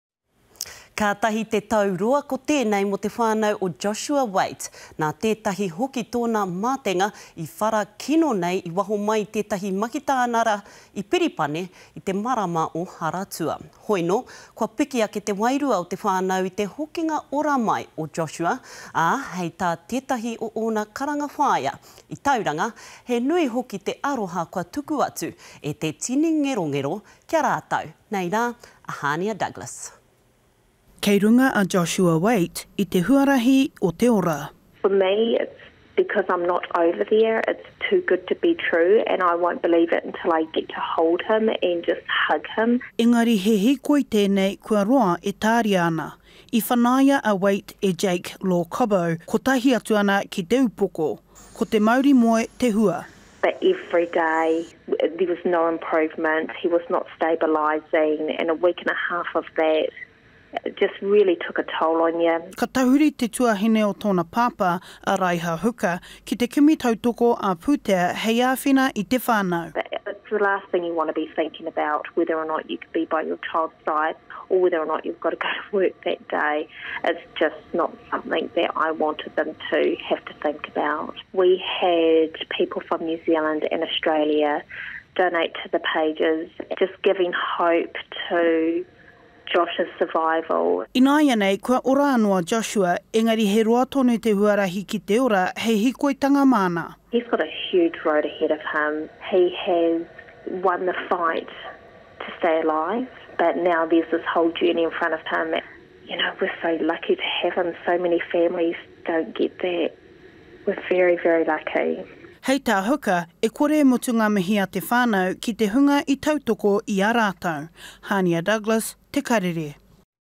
Aunty grateful for support given for nephew's recovery
It's been a long year for the family of Joshua Waite who was kicked in the head outside a Brisbane McDonald’s in May, but the family are now celebrating Joshua's recovery, and his aunty in Tauranga says the support they received from all over has been invaluable. Hania Douglas reports.